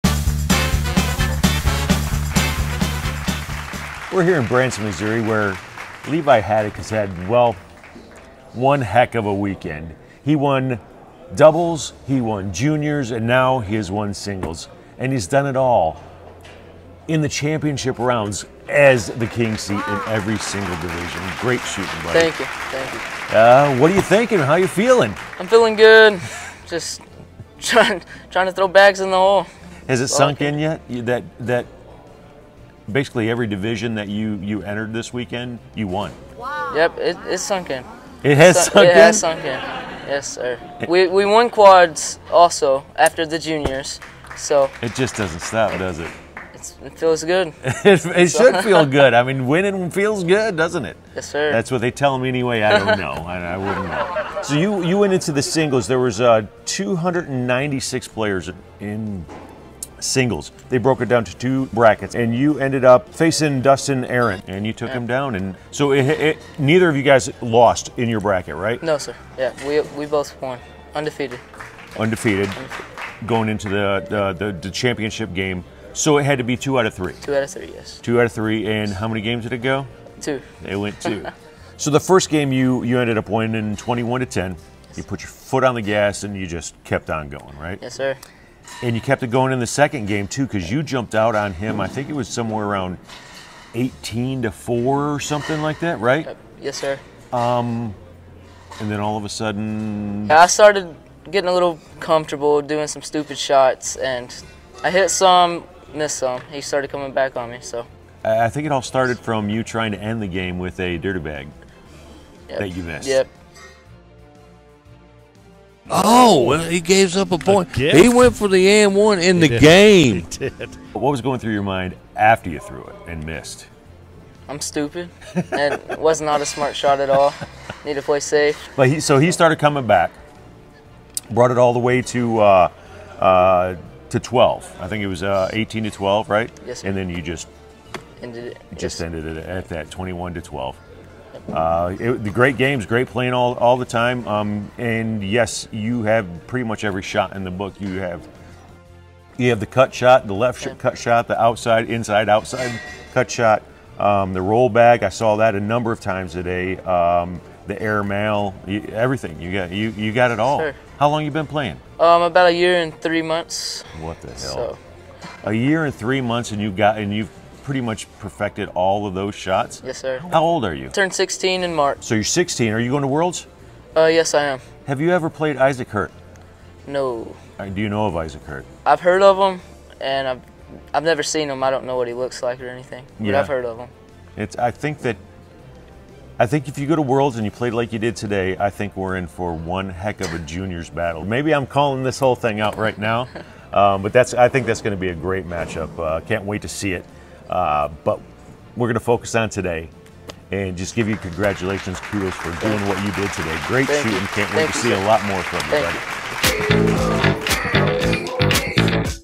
Levi Haddock, the winner of the ACO Branson Major Singles Division (among other things!)
It's a bird, it's a plane, nope, it's Levi Haddock, the Superman of the ACO Branson Major. If you didn't know his name before, you do now. Levi won Singles, Doubles, Juniors, and Quads and was in the King Seat for all!!! WOW!!! Congratulations, Levi! Great shooting!!